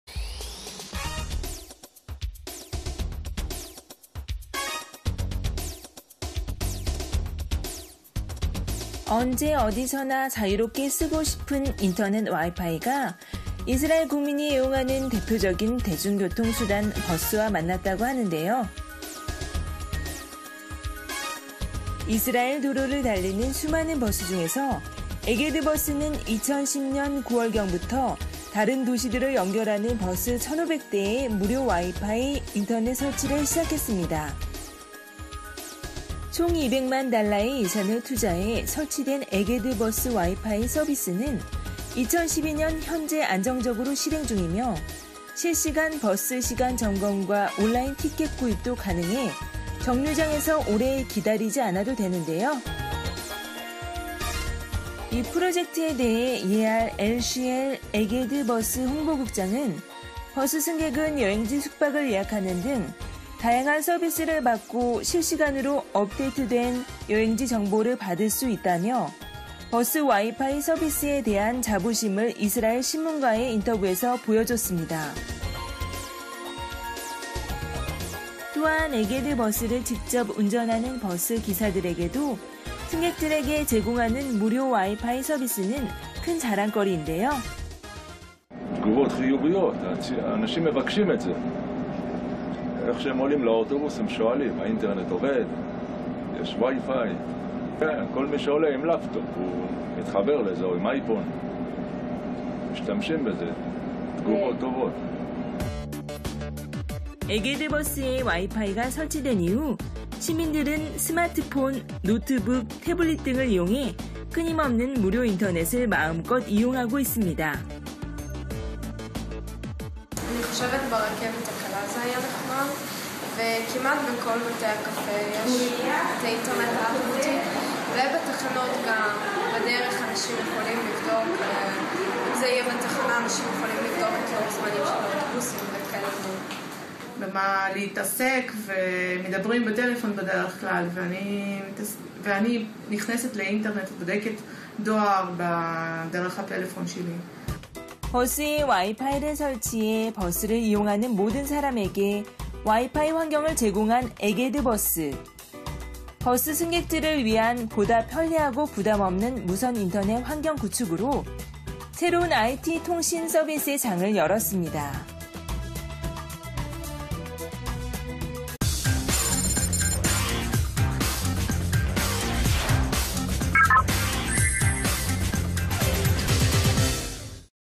버스 안 무료 와이파이가? 이스라엘 에게드 버스
무료로 와이파이를 제공하는 이스라엘의 에게드 버스를 만나보자